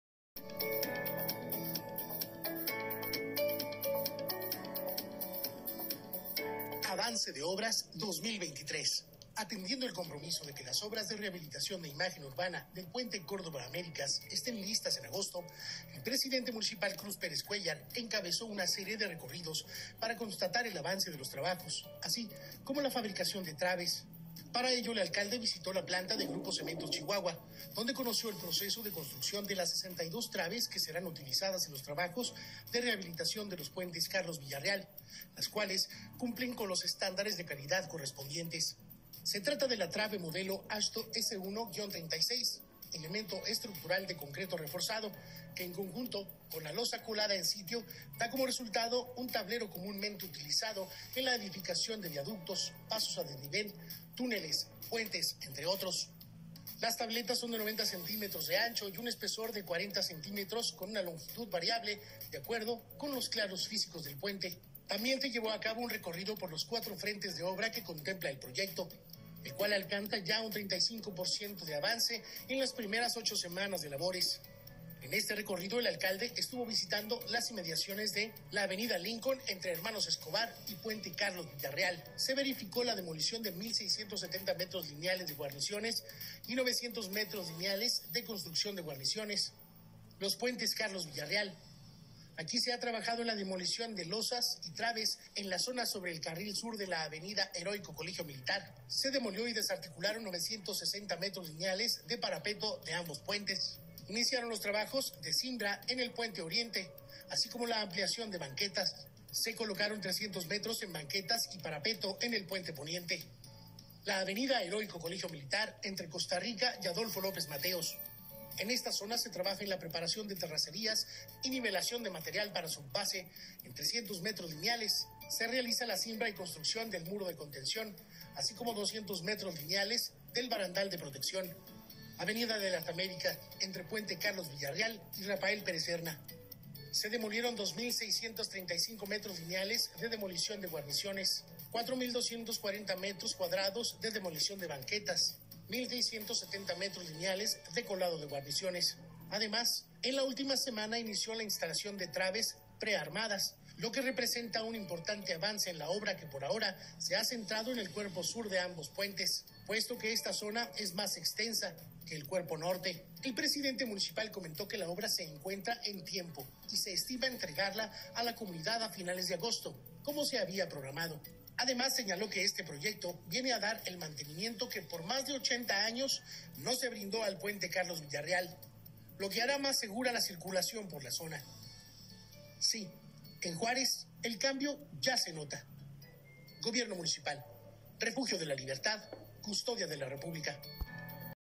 El Municipio Informó Avances de Obras 2023. Construcción/ colocación de Trabes P. Carlos Villareal
En este video la administracion municipal de Juárez informa sobre los avances en materia de obras públicas viales.
Entérese de la RehabilitacionPuenteCarlosVillarreal